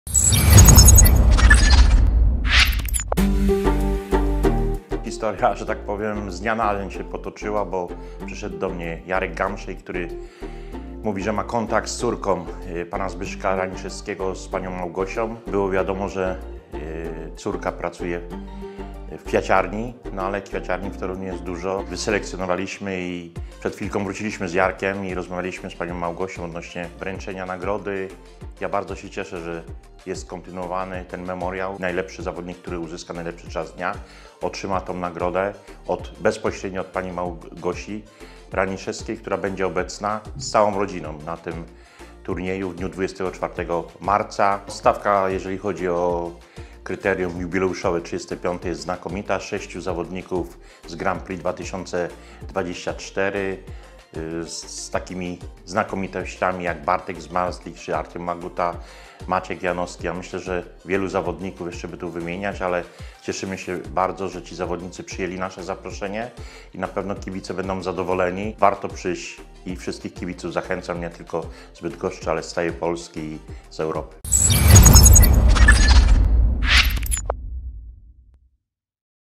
JERZY KANCLERZ O KRYTERIUM I MEMORIALE Z.RANISZEWSKIEGO
Prezes ŻKS Polonia Bydgoszcz SA - Jerzy Kanclerz o XXXV Kryterium Asów i XX Memoriale Zbigniewa Raniszewskiego.